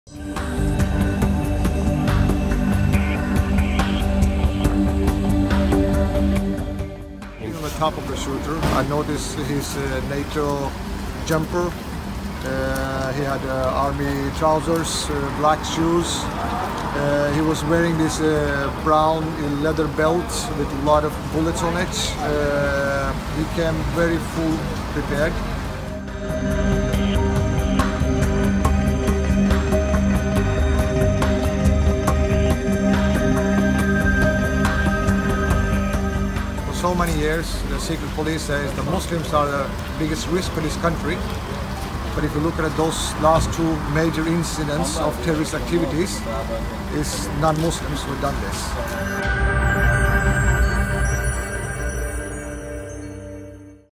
Norwegian police investigate mosque shooting as an 'act of terror'
A lone gunman opened fire in a Norway mosque on August 10. The attack is being investigated as a possible act of “terrorism.” A 65-year-old member of the mosque had overpowered the gunman before police arrived